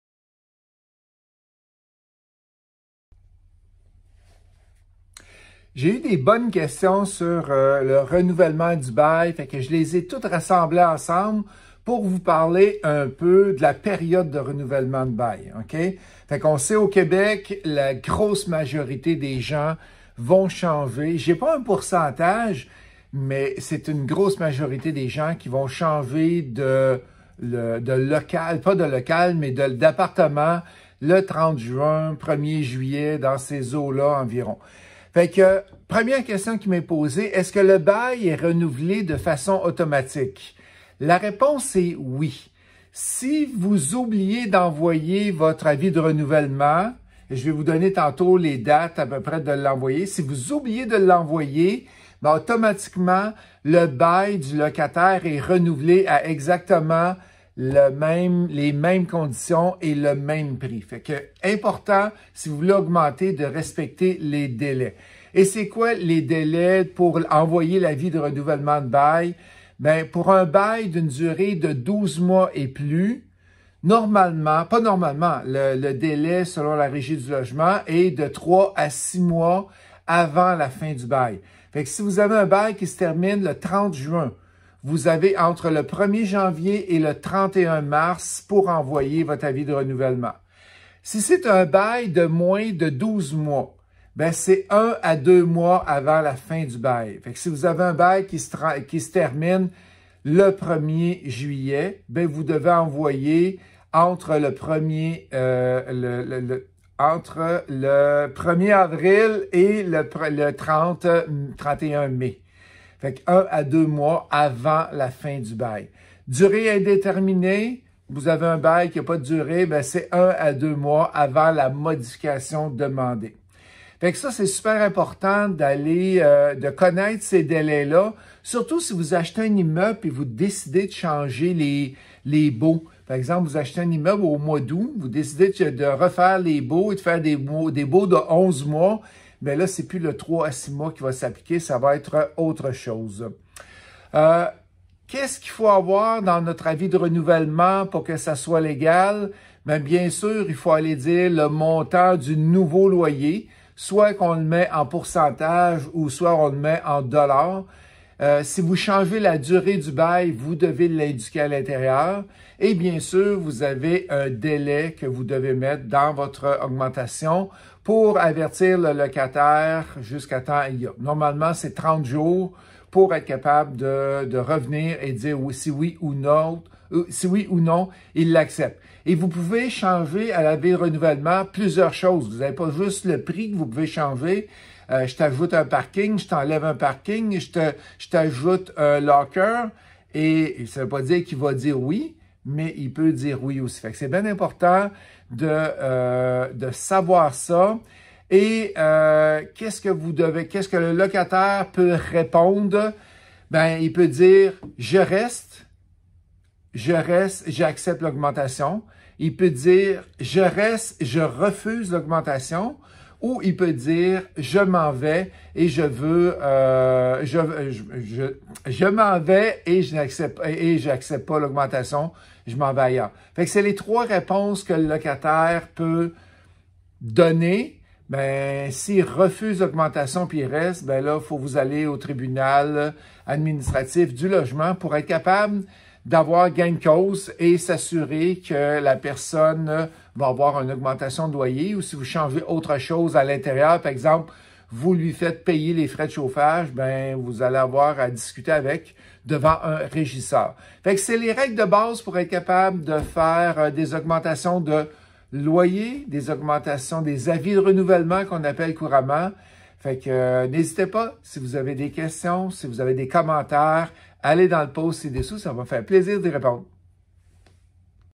Renouvellement bail (C'est le moment)!
Renouvellement bail (C'est le moment) - Vous avez des immeubles, des logements?  C'est le temps de penser à l'envoi de vos renouvellements de baux.
Quel délais? Quel pourcentage? Comment procéder? Voyez les conseils et infos de Ghislain.

▬▬▬▬▬▬▬▬▬▬▬ CONTENU GRATUIT ▬▬▬▬▬▬▬▬▬▬

Webinaire “Comment faire votre premier deal en moins de 90 jours”

▬▬▬▬▬▬▬▬▬▬  NOS FORMATIONS   ▬▬▬▬▬▬▬▬▬▬▬

▬▬▬▬▬▬▬▬▬▬▬▬ SUIVEZ-NOUS  ▬▬▬▬▬▬▬▬▬▬▬▬▬